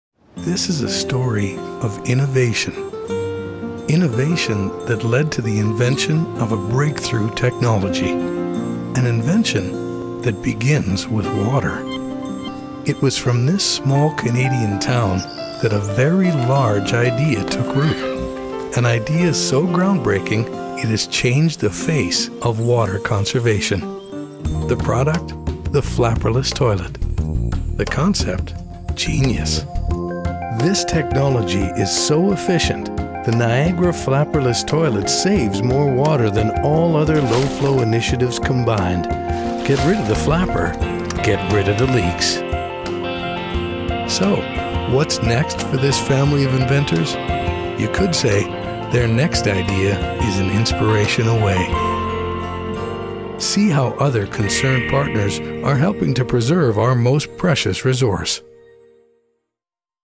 Niagara Flapperless - Water for Life
A TV SPOT in a series for the United Nations Canada Water for Life initiative. Visit thinkwater.ca for more information.

Innovative ideas from one family creates solutions to the water conservation issue.